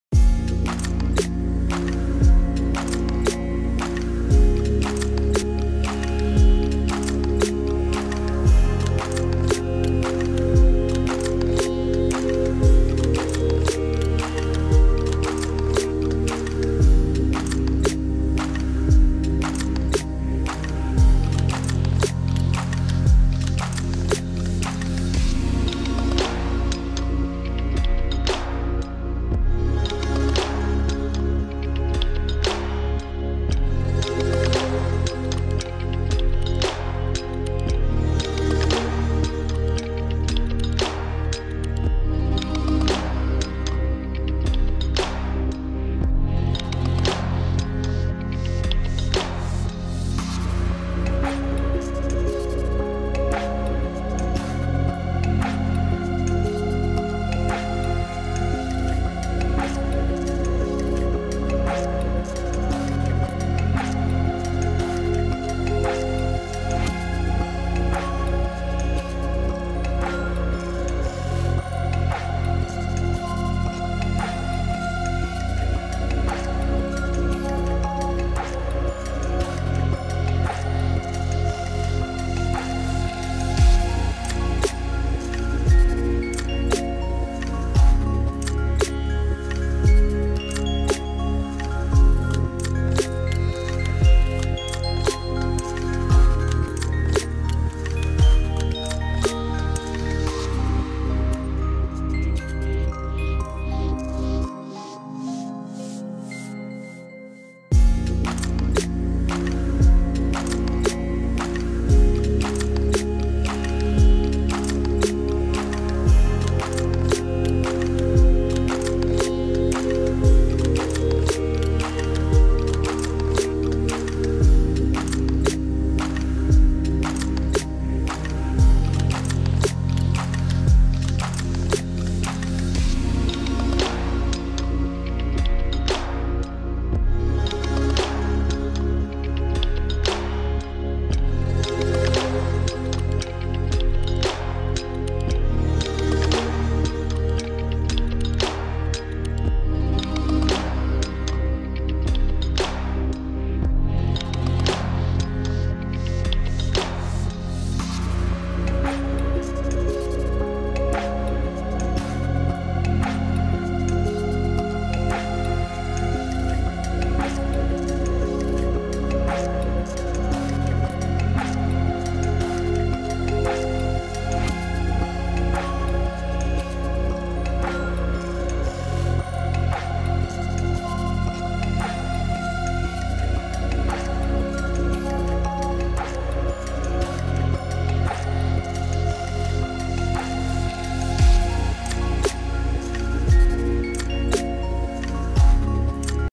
Oppo Reno 7 Cover & Case MEGA UNBOXING | Mobile Gossips
Oppo Reno 7 Back Covers MEGA UNBOXING exclusive on Mobile Gossips!

⚡ Free and Fast Shipping all over Pakistan !